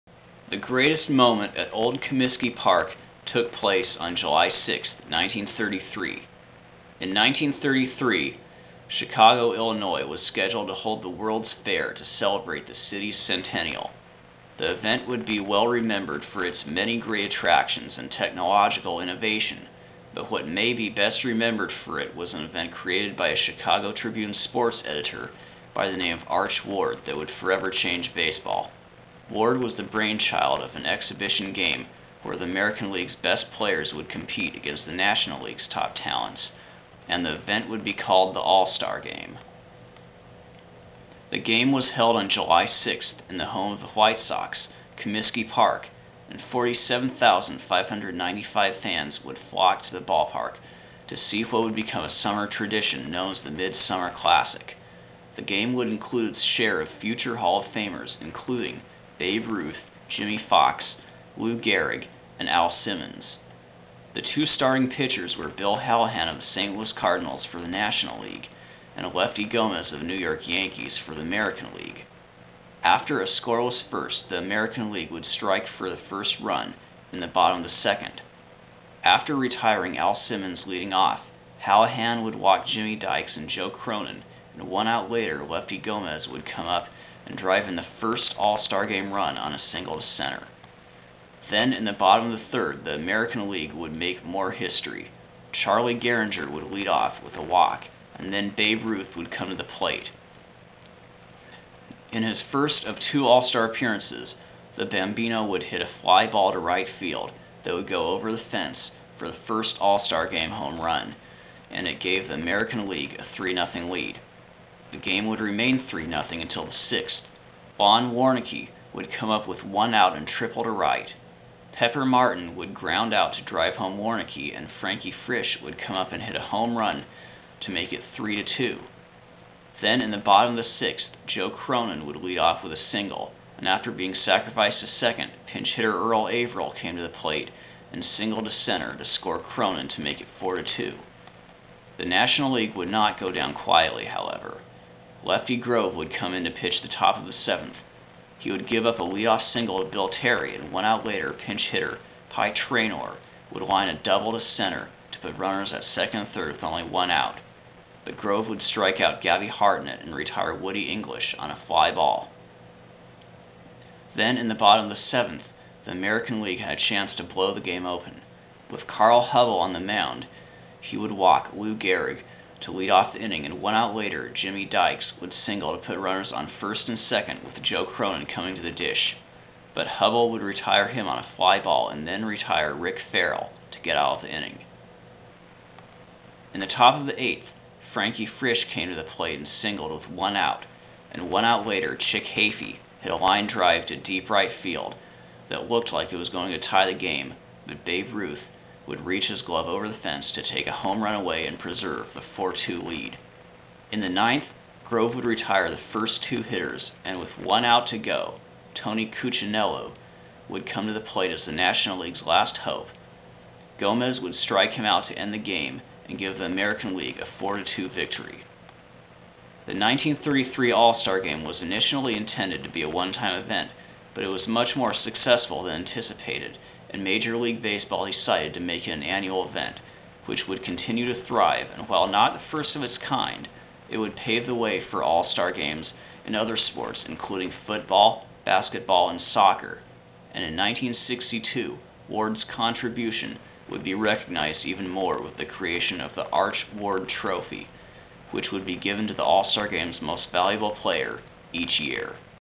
The greatest moment at Old Comiskey Park
The greatest moment at Old Comiskey Park is the 1933 All-Star Game.

Honorable mention:
1950 All-Star Game
Disco Demolition Night
1983 All-Star Game
1983 A.L. West clincher
Bobby Thigpen breaking the single-season save record